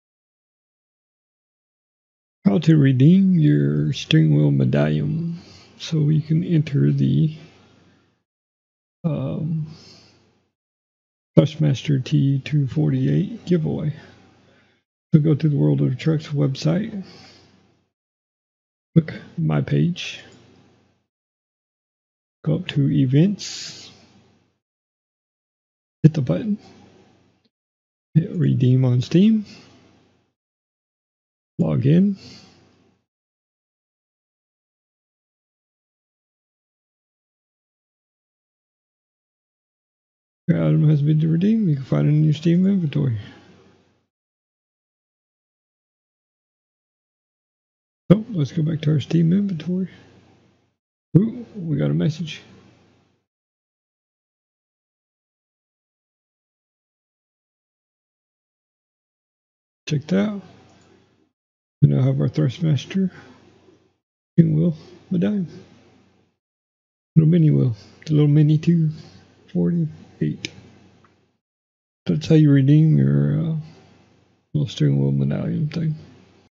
How to claim your steering wheel medallion |Free Wheel Give-a-way
How to claim your steering wheel medallion to enter the Thrustmaster t248 give-a-way good luck and safe trucking